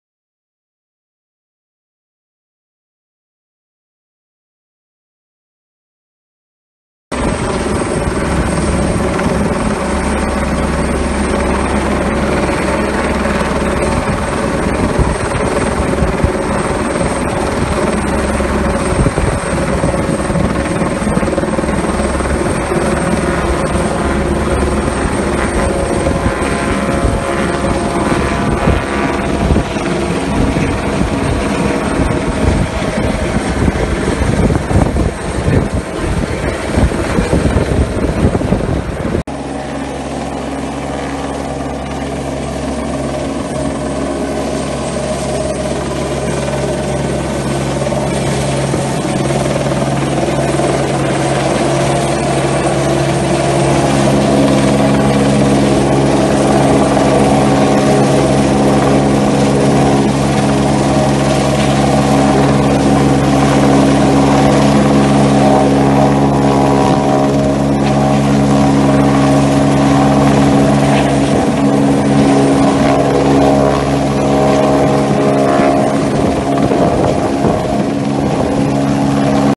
Hubschraubereinsatz beim SV Union Salzgitter
Um unsere neue Flutlichtanlage zu installieren, musste ein Hubschrauber geordert werden. Hier ein kleines Video dazu!